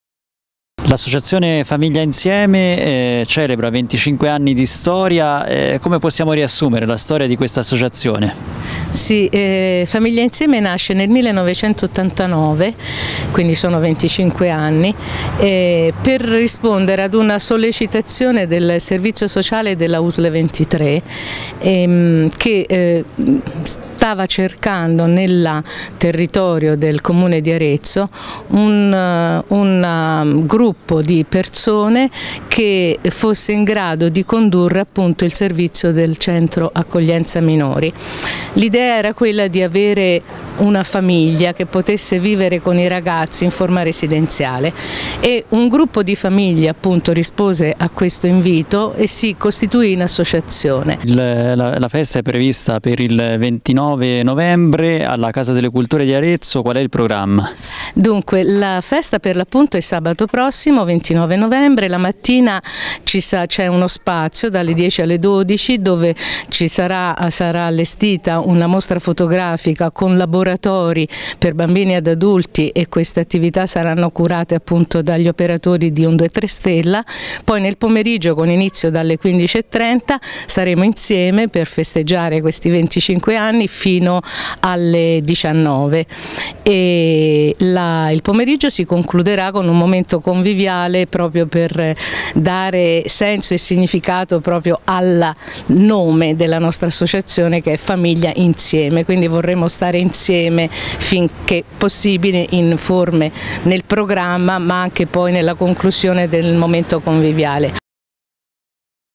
L'associazione famiglie insieme celebra 25 anni
L’associazione famiglie insieme di Arezzo taglia il traguardo dei primi 25 anni di attività.

Servizio di: Michele Francalanci
Giuliana Aquilanti - ass. Famiglie insieme